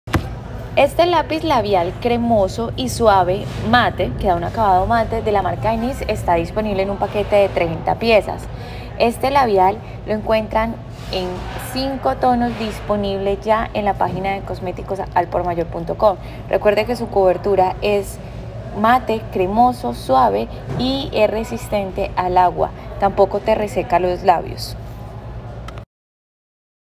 Cosméticos Crema Labial Variada Suave Mate NYX I Cosmeticos al por mayor
Crema Labial Variada Suave Mate Nyx

Crema de labios suave y mate: Cobertura mate cremosa y suave que nunca se siente seca, en desnudos mate y tonos rojos de lápiz labial inspirados en las ciudades del mundo. Deslízate sobre una crema de labios Soft Matte y transpórtate al instante!